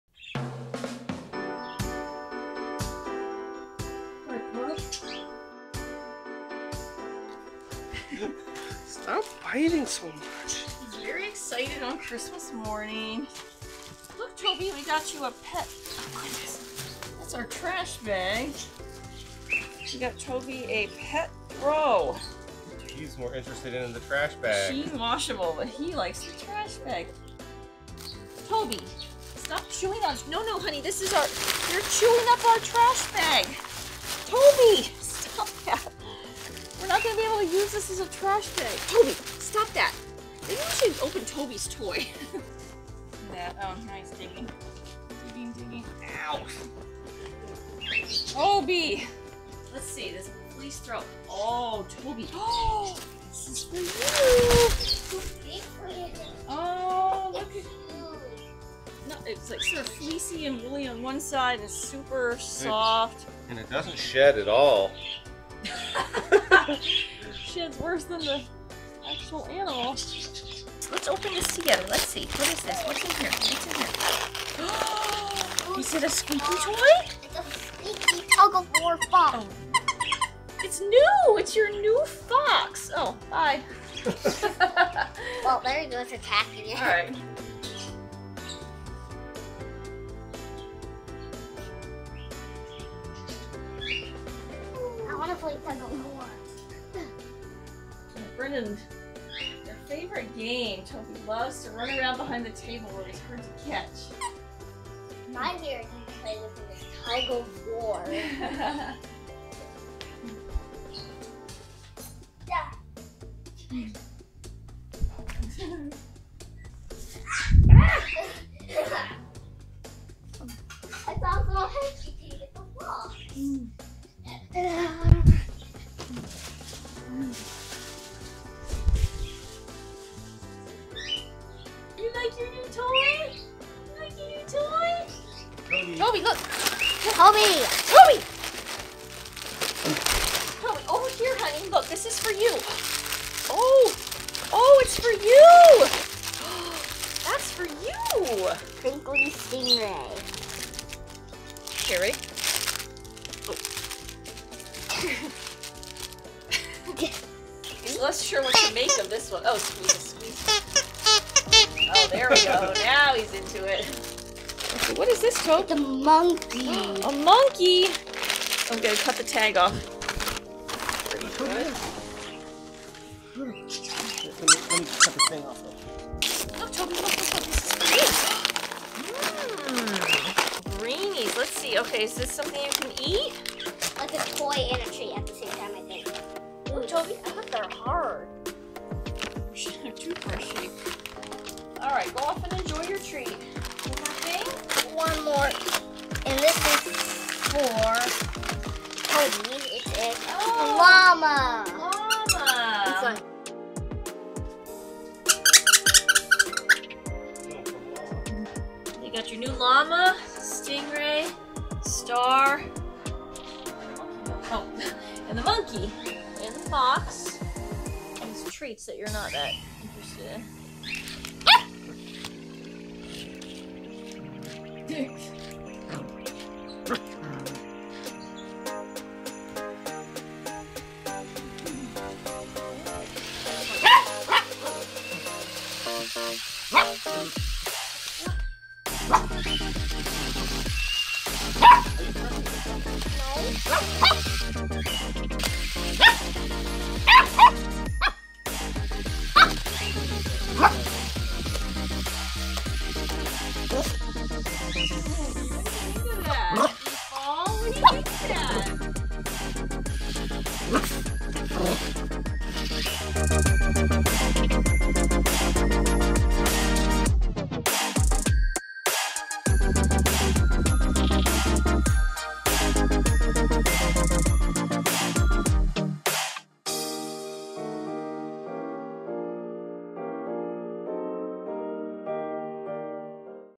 Puppy's First Christmas | Toby the Terrier Opens His Presents!
We celebrate our puppy's first Christmas at home and Toby the terrier and he gets some new toys and gifts!